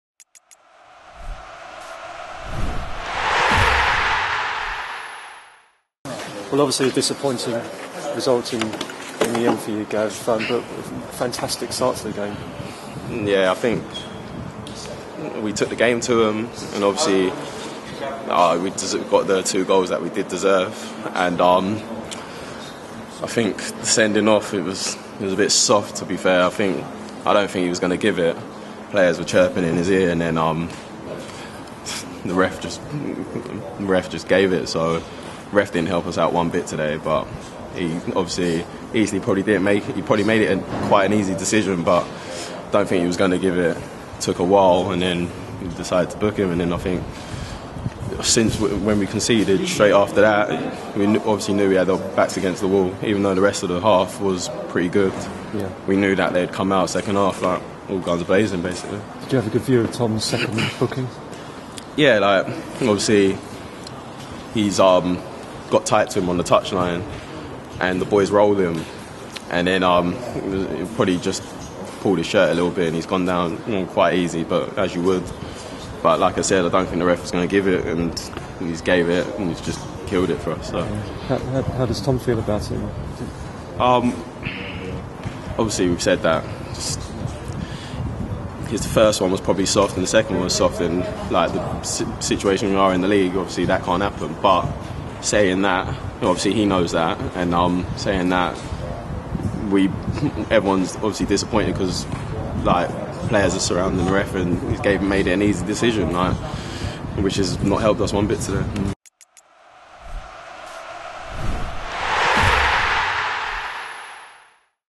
Massey Hurt By U's Defeat
Colchester United forward Gavin Massey reflects on the defeat at Bury